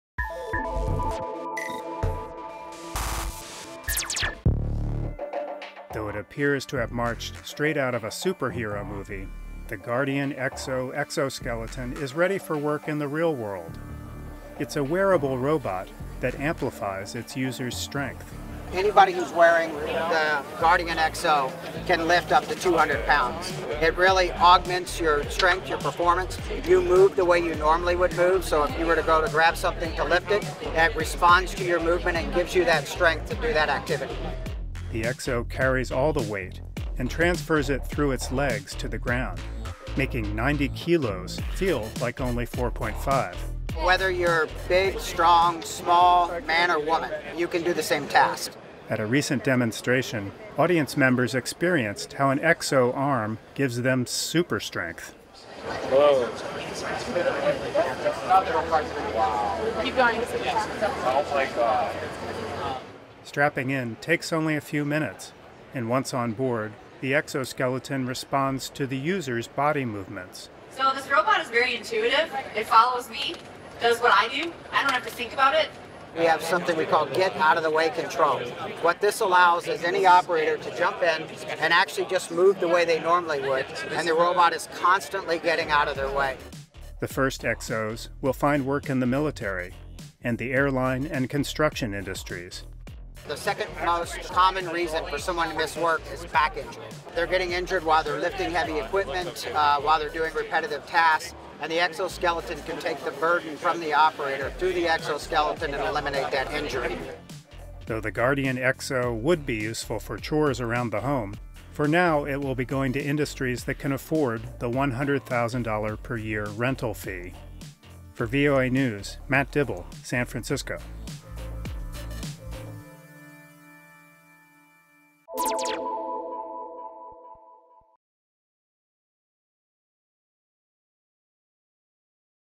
A Stronger Skeleton: Wearable Robot Gives Users Super Strength
No longer just for insects, humans are also donning exoskeletons to tackle heavy tasks